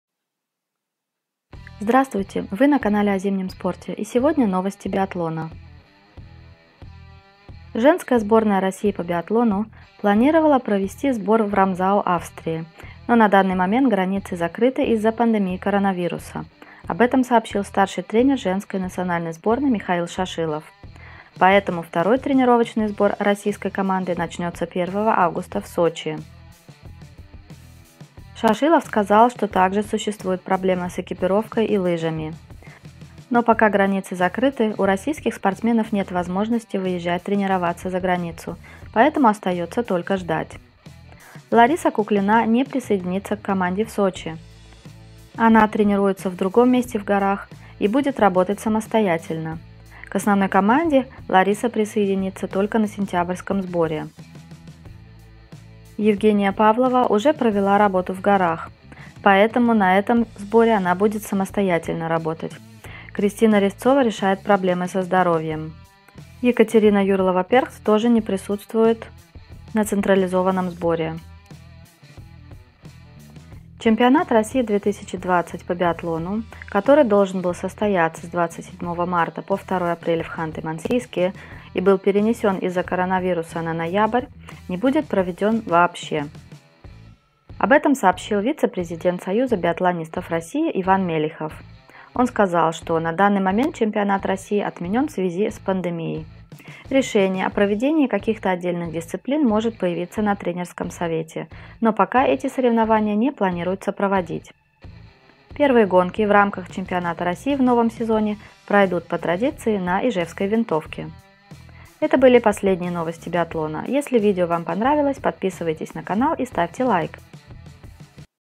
Тренер женской сборной по биатлону Шашилов о втором тренировочном сборе
Женска сборная по биатлону должна была поехать в Рамзау, Австрия, но поскольку границы в Европу все еще закрыты, сборная остается тренироваться в России.